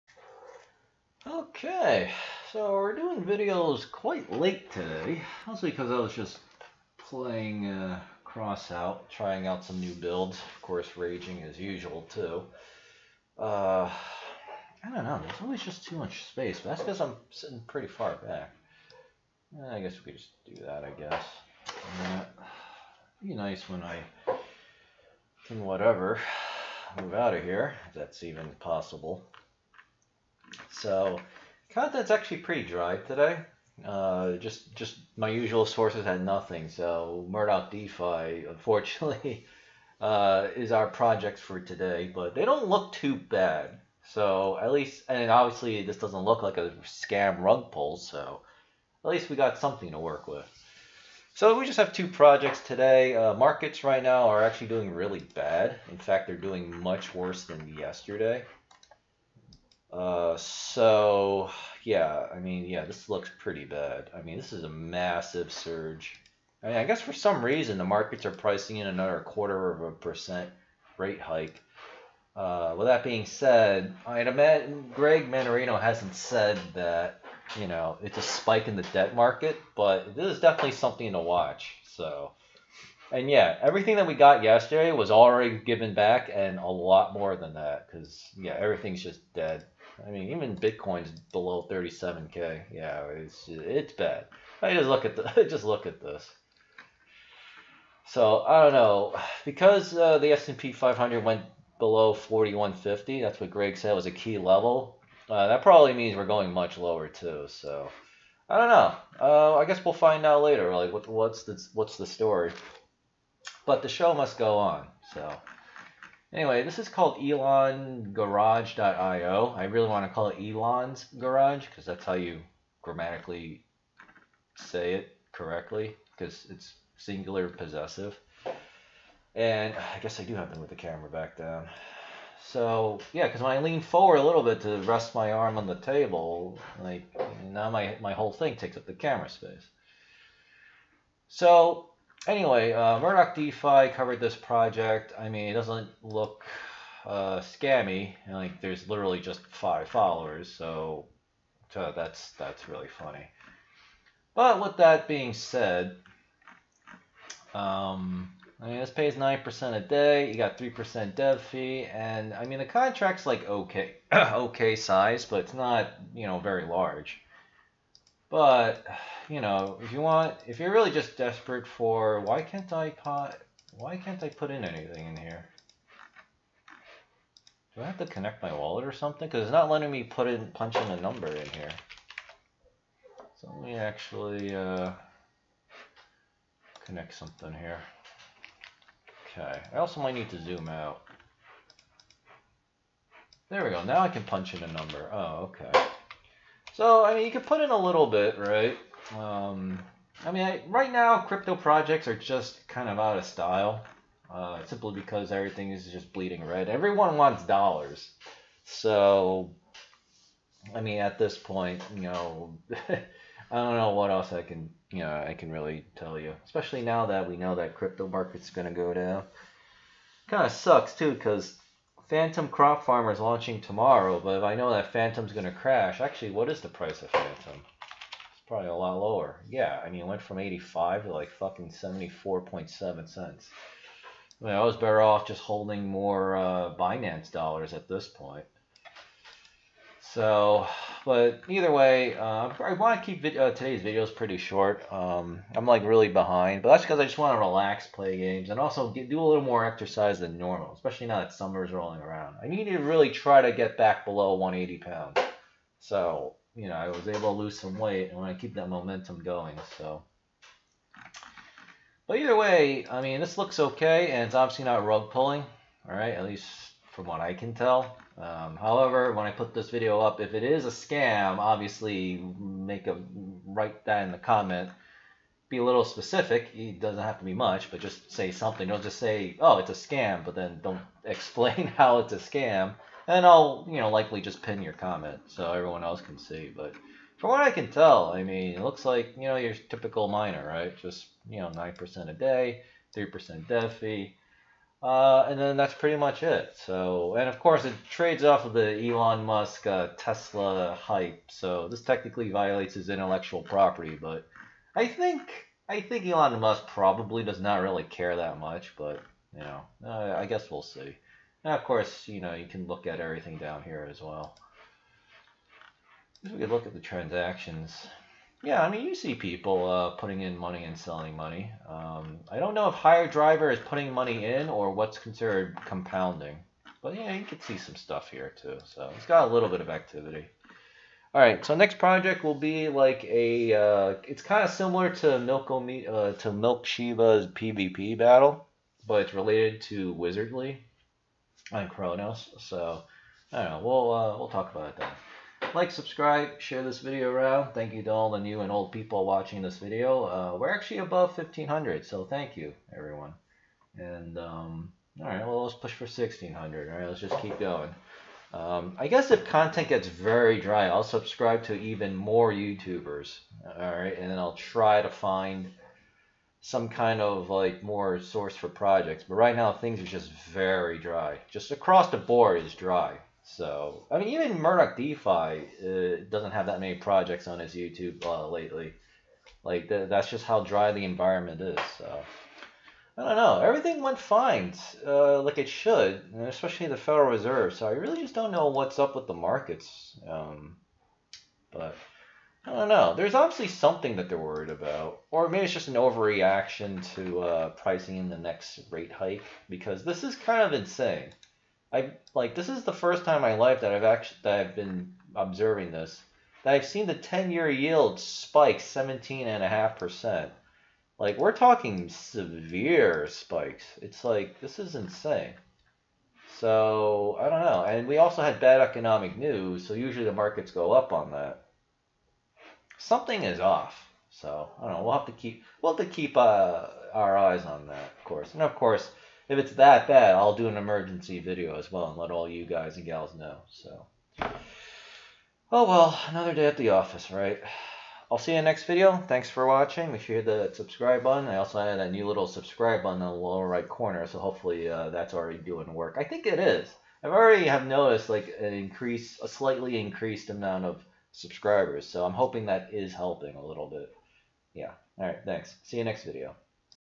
Elon Garage %9 Daily Apr Baked Beans BNB Miner Crops Farmer Fork - Earn Money Online Passively Cash
Pretty small project but looks like it's legit and is not rug pull scamming. Pays %9 daily and has a modest %3 tax. You probably have to size your investment to the lower size since the contract TVL is also small but looks consistent at least.

For real time crypto news and geopolitical risk updates, following my twitter